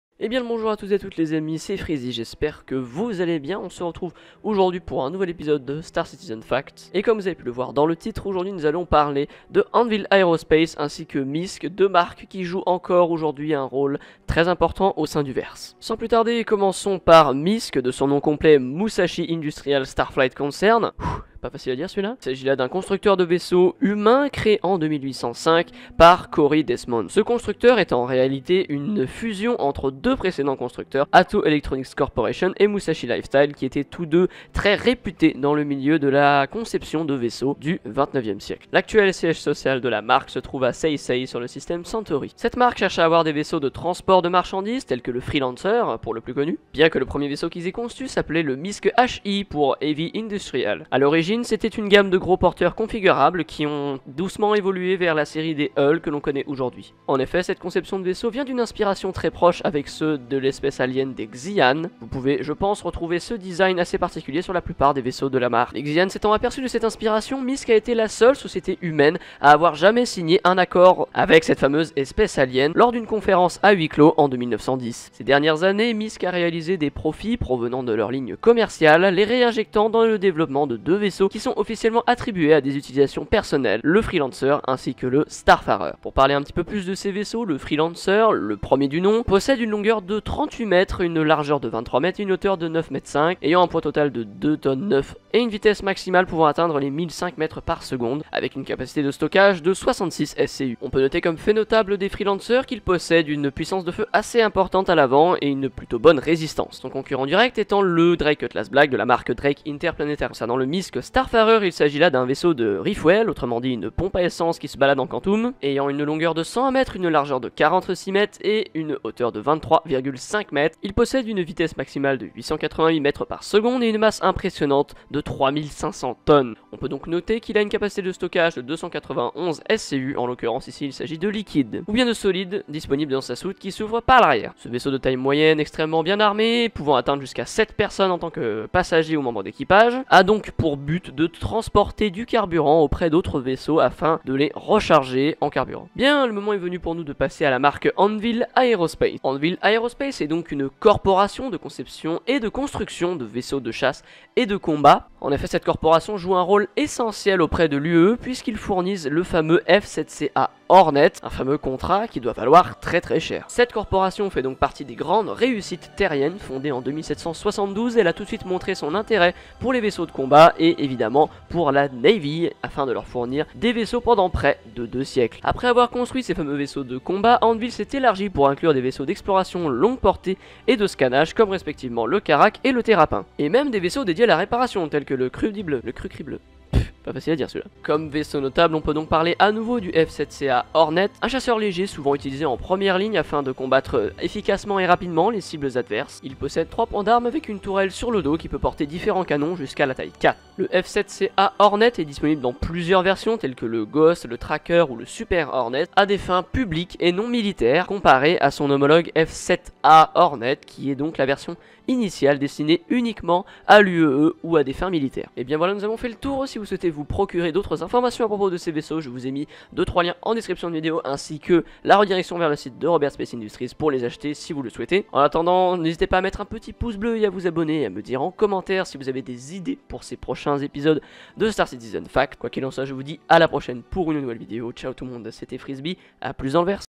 MISC ET ANVIL !
Salut ! Aujourd'hui, dans ce nouvel épisode de Star Citizen Facts, nous allons parler de MISC ainsi que de Anvil Aerospace !